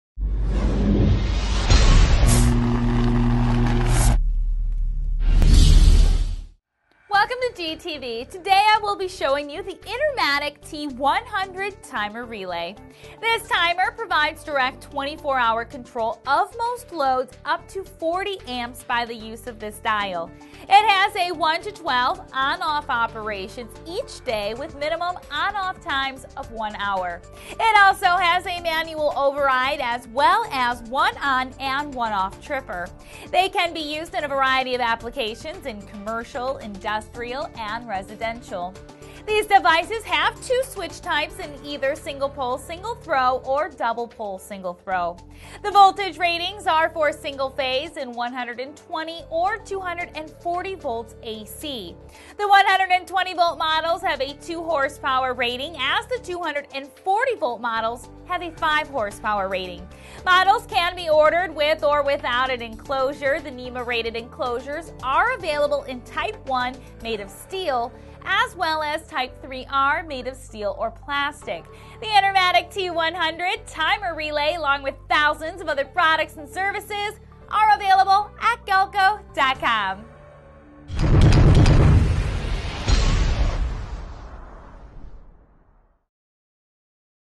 Intermatic T100 Timer Relay
Intermatic T100 Timer Relay presented by Katie Rydzewski for Galco TV.

Intermatic T100 Features:
-    Electrical loads up to 40 Amps
-    Provides direct 24 hour control of most loads
-    1 to 12 ON/OFF operations each day with minimum ON/OFF times of 1 hour
-    Manual override
-    All models equipped with one ON and one OFF tripper

Applications — Commercial, Industrial, Residential:
-    Indoor/Outdoor Lighting
-    Post Lanterns
-    Signs
-    Pool Heaters
-    Filters
-    Hydro Chlorinators
-    Air Conditioning
-    Blowers
-    Electric Fences
-    Pumps
-    Heating/Ventilating Systems
-    Fans
-    Music and Signal Systems
-    Poultry Foggers
-    Conveyors